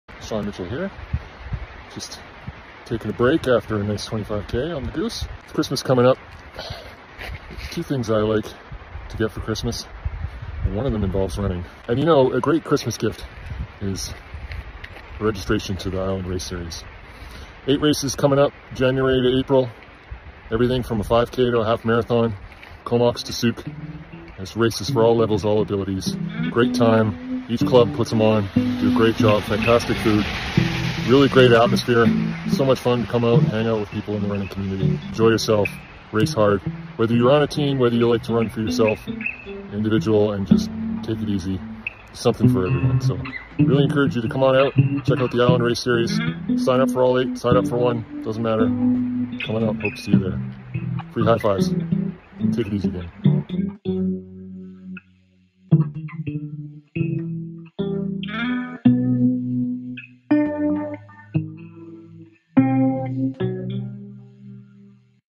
Meet 2024 series ambassador Sean Mitchell
Meet 2024 series ambassador Sean Mitchell who is representing the series for the eight races that will take place from Sunday, January 14 to Sunday, April 14.

He is the director of the Clover Point Park Run and board member of the Prairie Inn Harriers Running Club